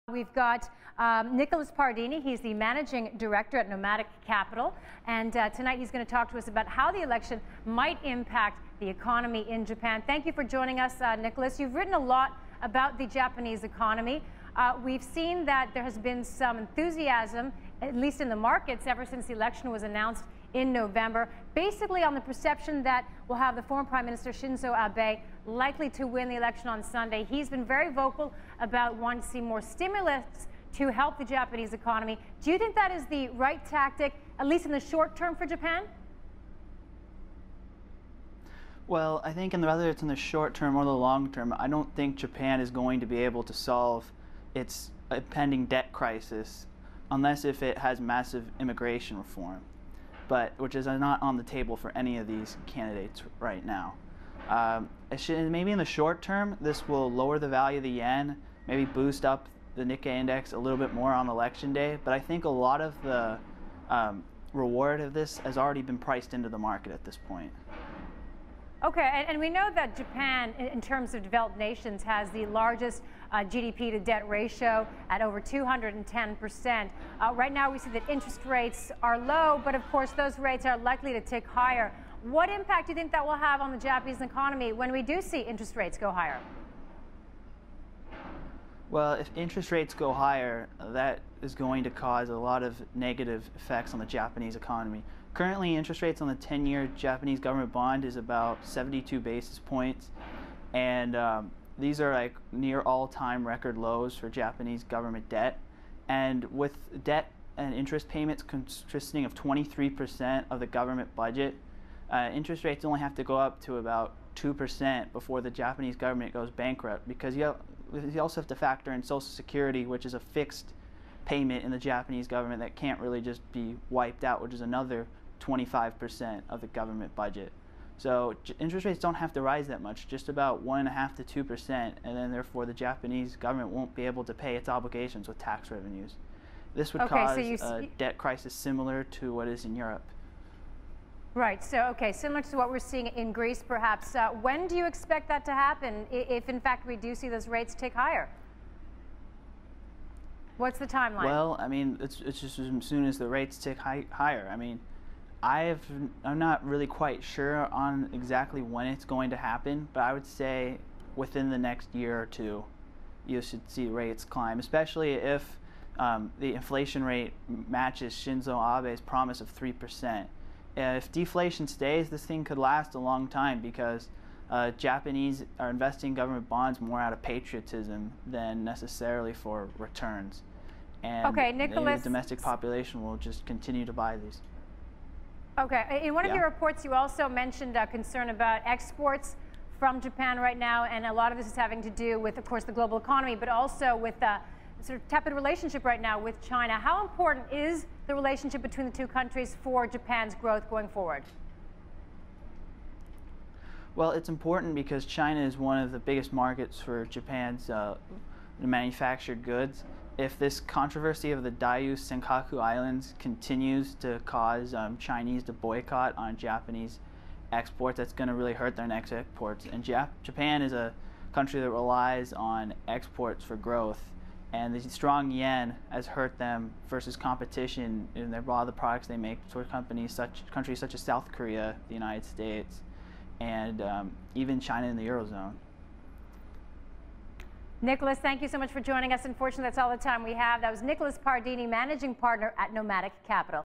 Nicholas Pardini on Japanese Economy
Nicholas Pardini, managing partner at Nomadic Capital Partners, discusses Japan's national election and its impact on Japanese economy.